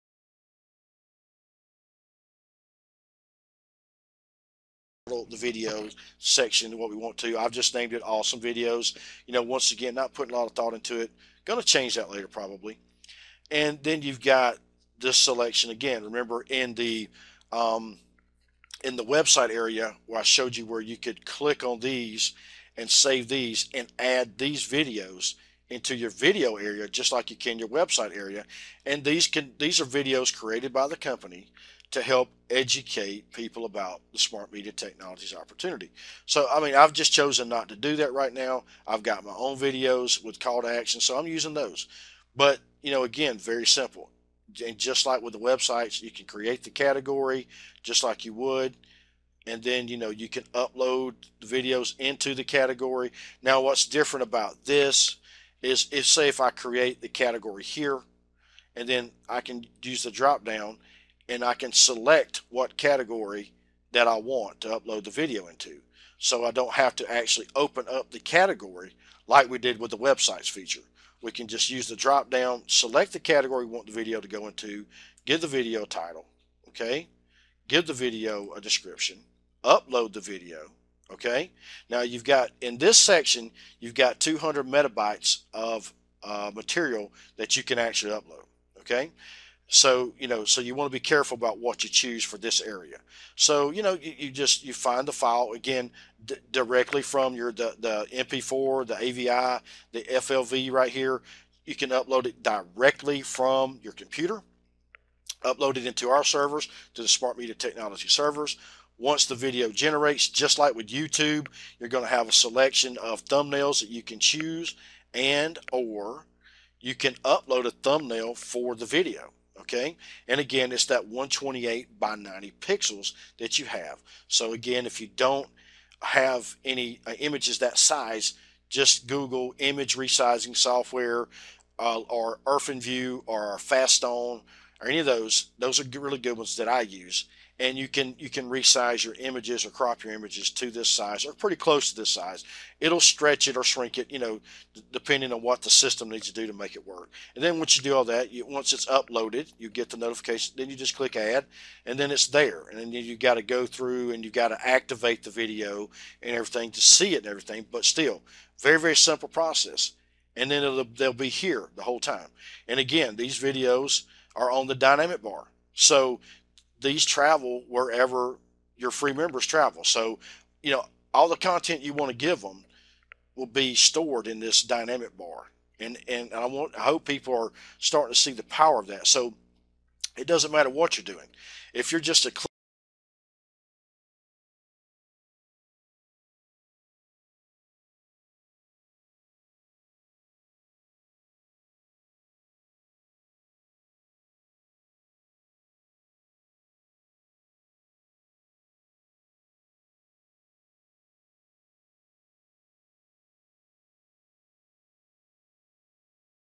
Smart Media Technologies Part 10
Smart Media Technologies

In this video series Steve Shoemaker will be discussing the features of the new social network YOBSN and how these features can be used by any business

or individual to market their product or services online.

This is the 10th video in the series.

Almost half-way through our in-depth overview of Smart Media and YOBSN. I hope you are beginning to see the great features and are seeing how this could apply to your business.

Smart Media Technologies is on the move and you need to tell people about it before someone tells you!